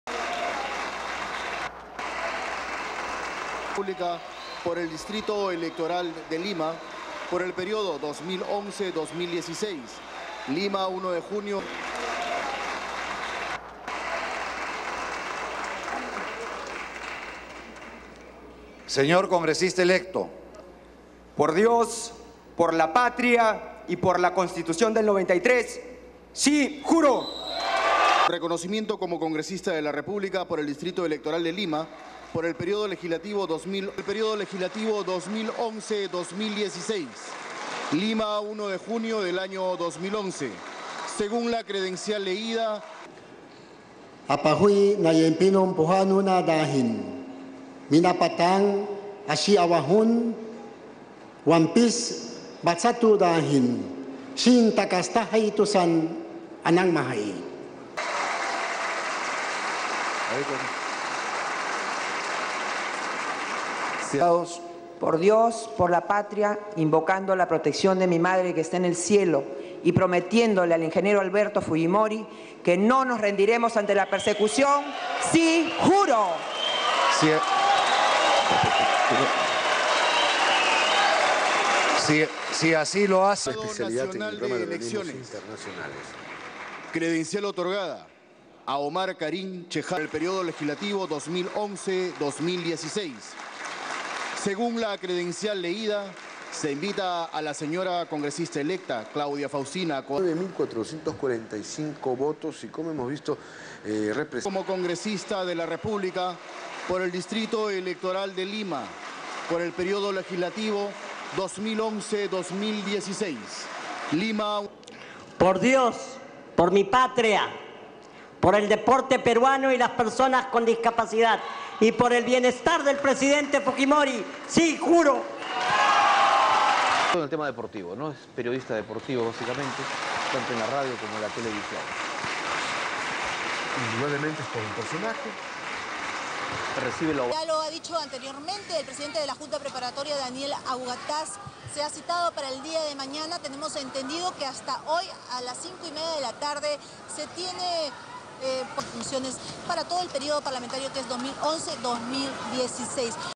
4:3 Swearing-in ceremony for new members of Congress
1. Wide shot Congress
2. Zoom in of elected congressman, and the youngest son of former president Alberto Fujimori, Kenji Fujimori walking to be sworn in
3. Fujimori preparing to be sworn in
4. SOUNDBITE (Spanish) Kenji Fujimori, Elected Congressman:
"For God, for the country and the constitution of 93, yes I swear."
5. Wide of congress
6. Zoom out of congressman from the Awajun tribe, Eduardo Nayap Kinin, at swearing-in
7. SOUNDBITE (Quechua) Eduardo Nayap Kinin, Elected Congressman:
"I thank the God of Jacob, who gives me the privilege to serve God at this occasion and to fight for the recognition of identity, of the human rights of the indigenous, who were largely ignored in the constitution of the republic and constitutional rights."
8. Medium shot congress members applauding
9. SOUNDBITE: (Spanish) Martha Chavez, Elected Congresswoman:
"For God, for the homeland, I am invoking the protection of my mother who is in heaven and promise to Alberto Fujimori that we will not surrender to the persecution, I swear."
10. Wide of Congress
11. Zoom out Claudia Faustina Coari, Congresswoman from Quechua indigenous group, walking to podium and greeting officials
12. Various of elected congressman Giancarlo Vacchelli
13. SOUNDBITE: (Spanish) Giancarlo Vacchelli, Elected Congressman:
"For God, for my homeland, for Peru's sport, for handicapped people, and for the well-being of President Fujimori, I swear."
14. Various of Vacchelli in wheelchair saluting people while others applaud
15. Various of congressmen and congresswomen with their medals after being sworn in
STORYLINE:
Peru's newly-elected congressmen and congresswomen were sworn-in on Monday, three days before Ollanta Humala assumes the presidency.
130 new representatives were sworn in on Monday including Kenji Fujimori, the youngest son of former disgraced president Alberto Fujimori, who made the point of swearing to the "the constitution of 1993", introduced by his father who was president from 1990 to 2000.
The wording was a thinly veiled swipe at Humala who has said changing the constitution would be one of the goals of his time in office.
Others were also vocal of their support for the former leader including congresswoman Martha Chavez whose pledge to keep her "promise to Alberto Fujimori that we will not surrender to the persecution" was met by a mixture of cheers and boos.
The swearing-in also showed the diversity of Peruvian society with indigenous congressmen and women taking their oaths.
"I thank the God of Jacob, who gives me the privilege to serve God at this occasion and to fight for the recognition of identity, of the human rights of the indigenous, who were largely ignored in the constitution of the republic and constitutional rights," said  Eduardo Nayap Kinin, a congressman from the Awajun tribe.
One of the largest welcomes was reserved for handicapped congressman Giancarlo Vacchelli, who received a standing ovation from his colleagues as he took his oath from a wheel chair.
"For God, for my homeland, for Peru's sport, for handicapped people, and for the well-being of President Fujimori, I swear," Vacchelli said.